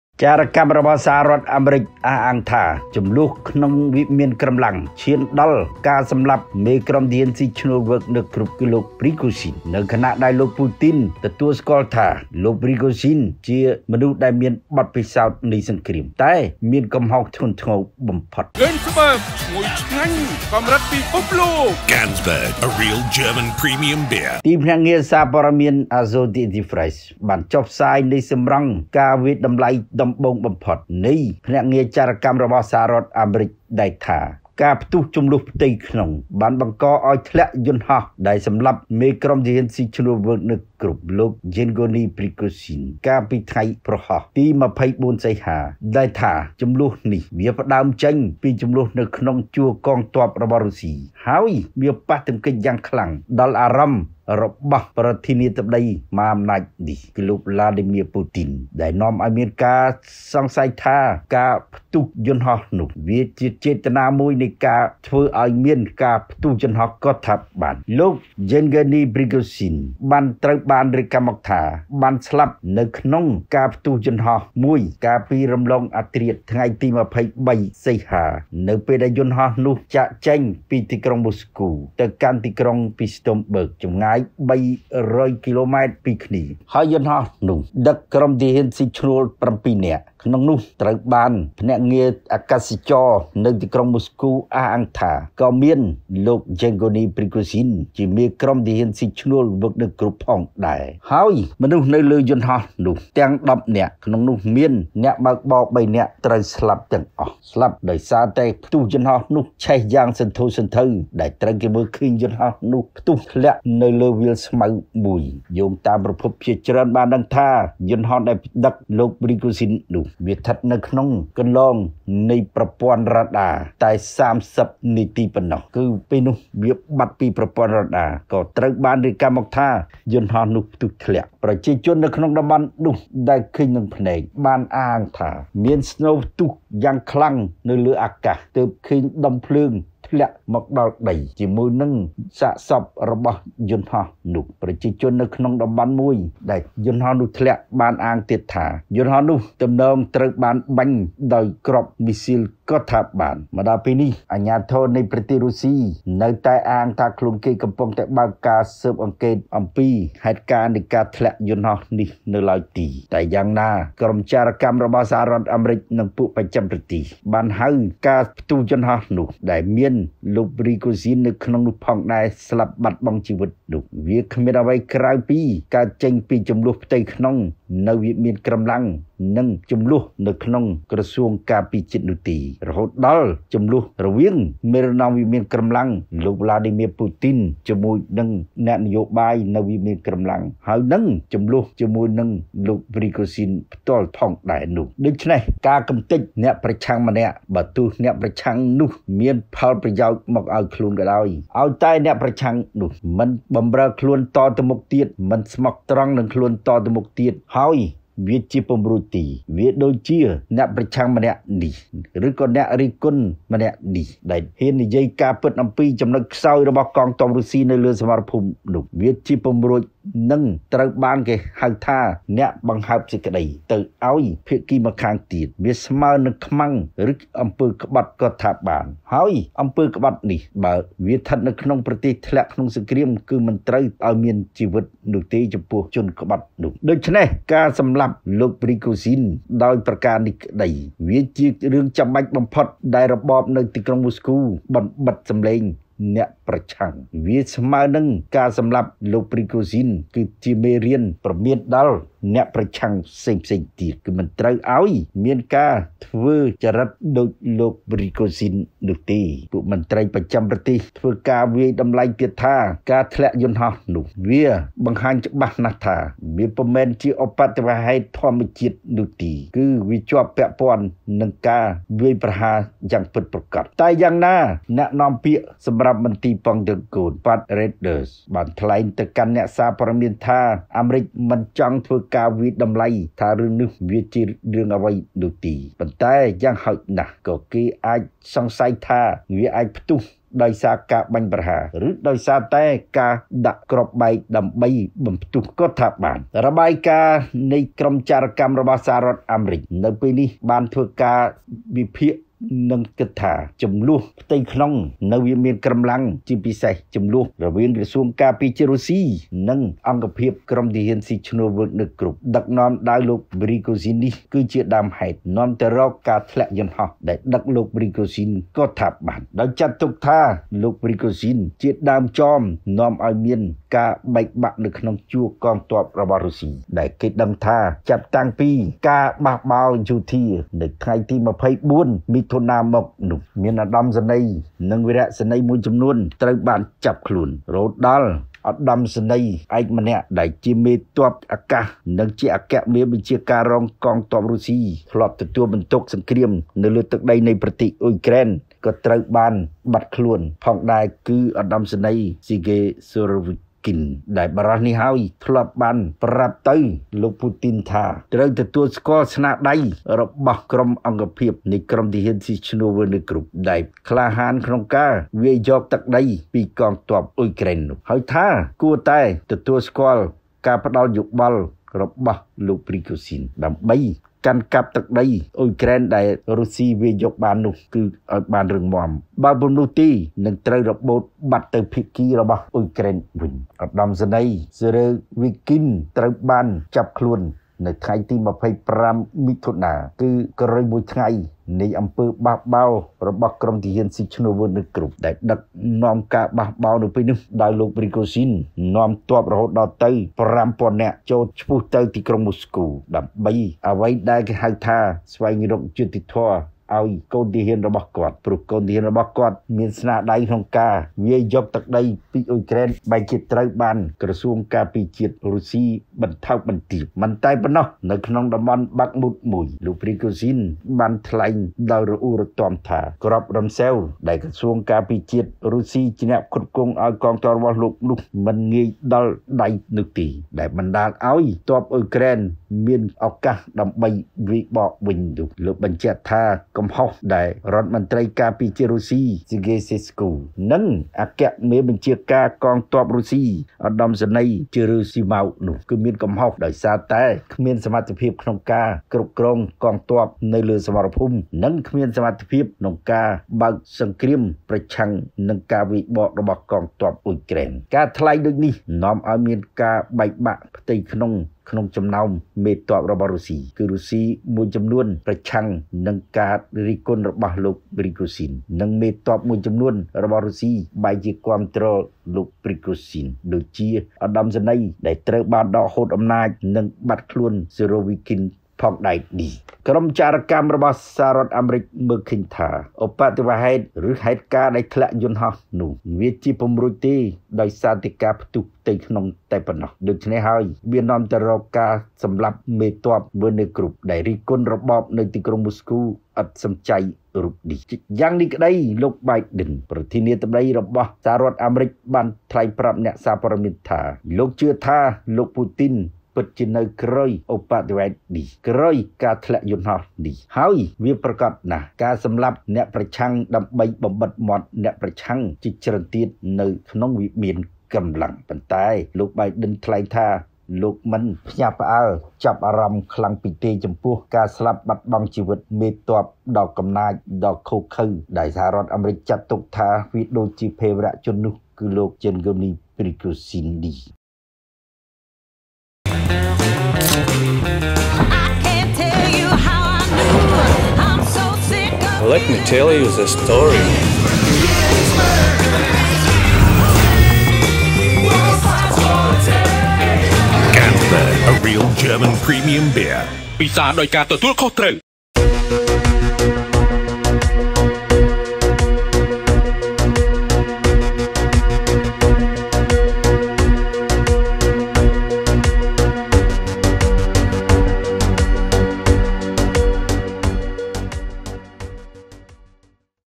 ចារកម្មអាមេរិកថាជម្លោះក្នុងវិមានក្រឹមឡាំងឈានដល់ការសម្លាប់មេក្រុមទាហានស៊ីឈ្នួល Wagner
'We don't have any information to indicate right now that there was some surface to air missile that took down a plane.'Pentagon Press Secretary Patrick S. Ryder speaks on the plane crash near Moscow that repordedly ended Prigozhin's lifeSky News